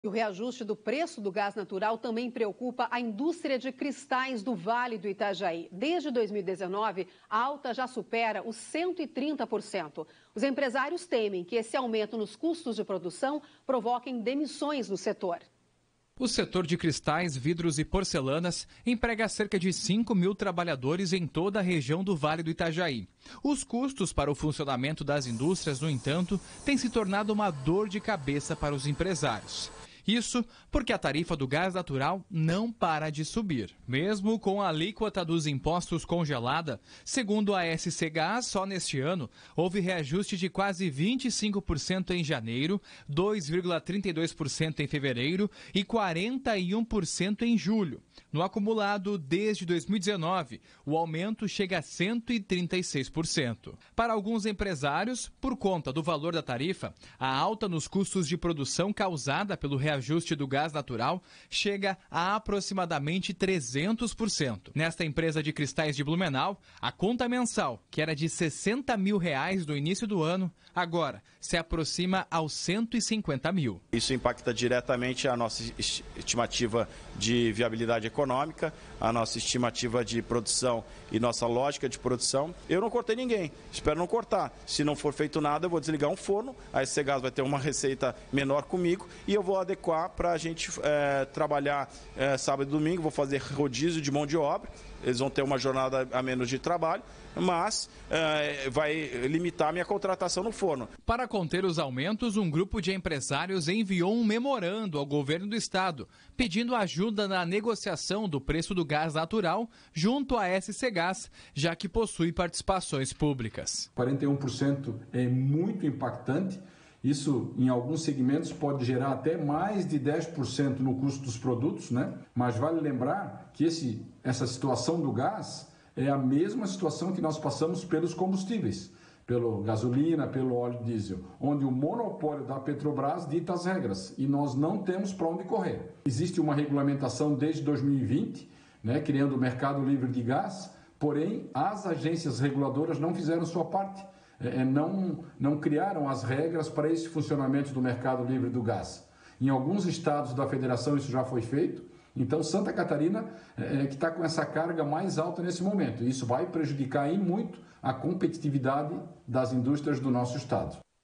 Aumento na tarifa do gás natural preocupa indústrias de cristais
A tarifa do gás natural teve um aumento de 136% desde 2019. Esse fator eleva o custo de produção da indústria de cristais de Santa Catarina.

Você encontra todas as matérias do ND Notícias na área especial do portal ndmais.com.br para os conteúdos da NDTV